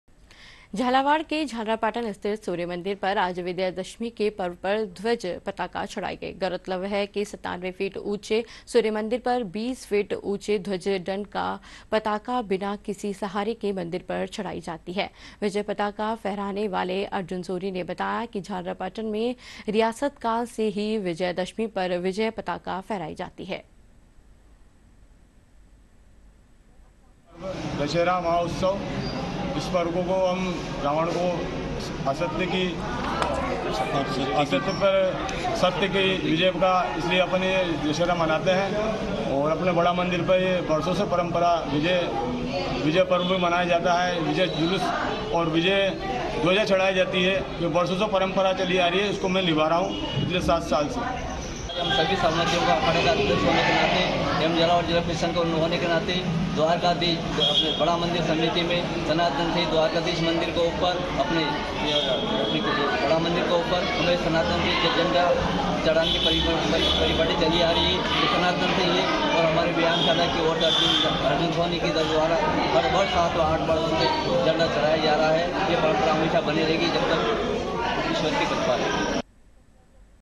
Jhalawar : सूर्य मंदिर के शिखर पर लगाई नई ध्वज पताका, विजयादशमी पर सालों से निभाई जा रही परंपरा
जिले के झालरापाटन स्थित अति प्राचीन सूर्य मंदिर पर विजया दशमी के मौके पर वर्षों पुरानी परंपरा के मुताबिक नई ध्वज पताका स्थापित की गई। ध्वज पताका को फहराने के लिए पहले विधि-विधान से पूजन किया गया। उसके बाद शहर के अर्जुन सोनी ने बिना किसी अतिरिक्त सुरक्षा संसाधनों के मंदिर शिखर तक चढ़कर धार्मिक ध्वज पताका को स्थापित किया।